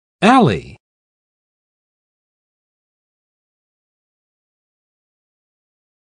How to Pronounce Alley
This video shows you how to say Alley.